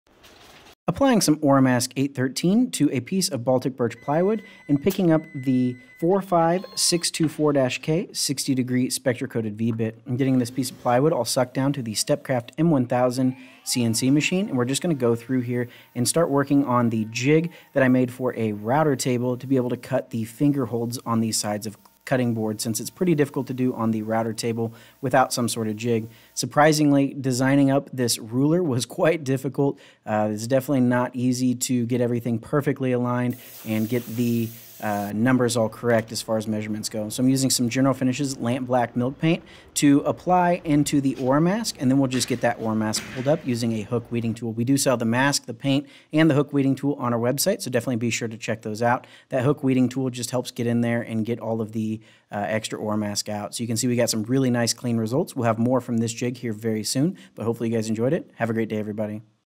Using Masking to Create a Ruler
Making a ruler for a router table finger hold jig using the Amana Tool item no. 45624-K Spektra coated 60 degree v bit using the Stepcraft M.1000 CNC machine, links to all tools used are down below.

Tools Used:

M.1000 Vacuum Table
Oramask 813
General Finishes Lamp Black Milk Paint
Amana Tool 45624-K SC Spektra Extreme Tool Life Coated V-Groove 60 Deg x 1/4 D x 31/64 CH x 1/4 Inch SHK 3-Flute Router Bit
Design Software - Vectric V-Carve Pro -